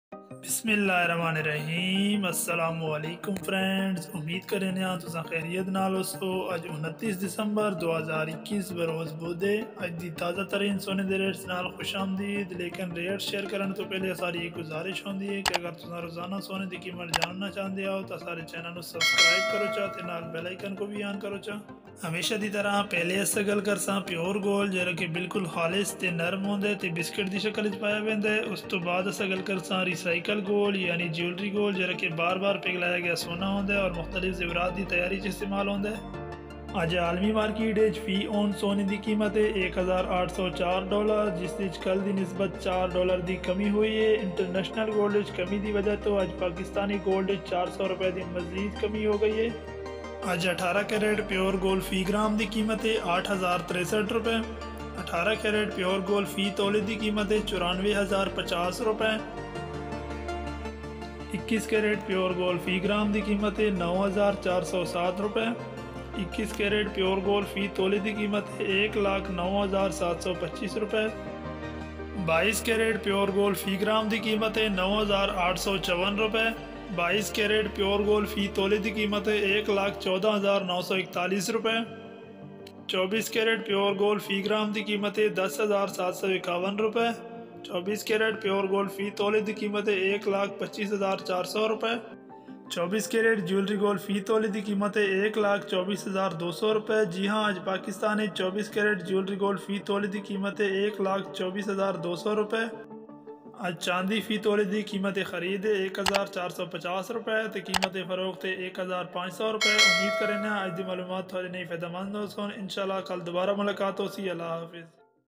Aj di taza tareen Gold Prices | Today Gold Rates in Pakistan | 29 December 2021 | Gul Gold News
Aj gold do Qimat vich zabadast kami
1tola gold.Prices in the form of 1 tola and 10 grams are paid in PKR Pakistani currency or Rs.These prices are straightforward, these prices are reported through the Pakistani currency markets, Pakistani cities, which include Karachi's big money market, Lahore, Rawalpindi, Multan, Peshawar and Quetta and other major cities of Pakistan.Gold prices vary in all cities of Pakistan, we have given gold prices according to all cities of Pakistan. The global billion market rate is determined by PSX, and we will give you the details.Gold in Pakistan comes mainly from the markets of Dubai, Saudi Arabia, and prices cannot be estimated due to currency factors in Pakistan.However you may have the best guess.